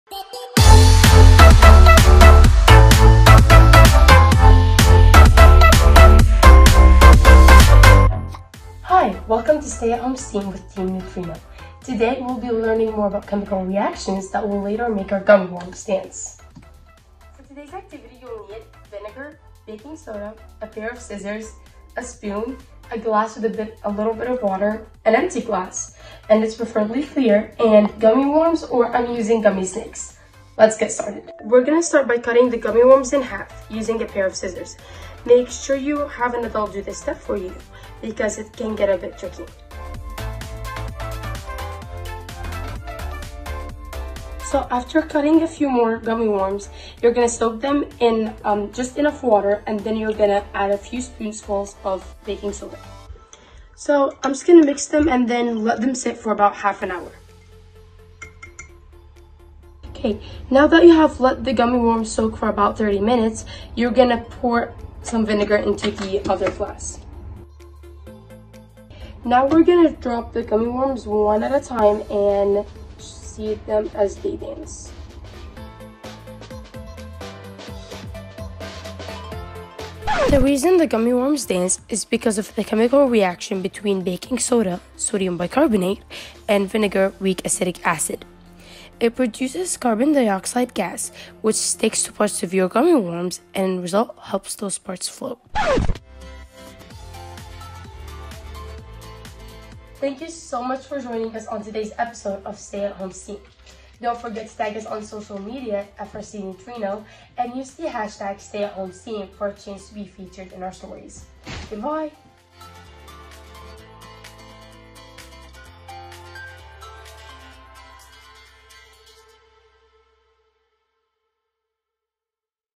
Stay-At-Home STEAM: Dancing Gummy Worms!
Talk with an adult before trying the activity. For this video, you'll need:

Vinegar, Baking Soda, Scissors, a Spoon, a Glass of Water, an Empty Glass, and Gummy Worms. Enjoy!

Edited by Quinn Margrett